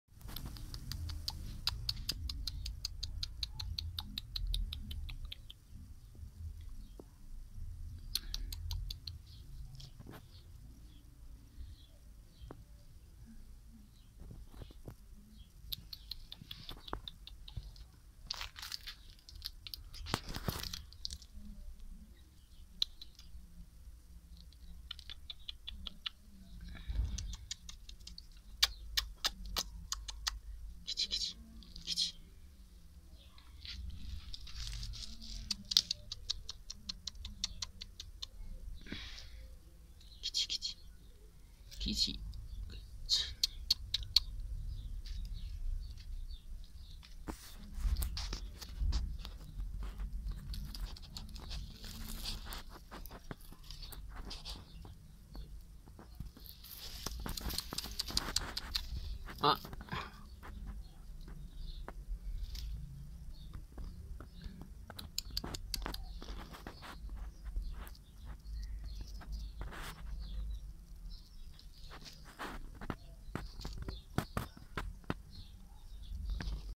A wonderful day a black cat has been stroked on🎩🎩🎩
Hello friends, I wish you a beautiful day with health and wellness. I am very beautiful animals, please take care of them🍯🍯🍯🍯🍯🍯🍯🍯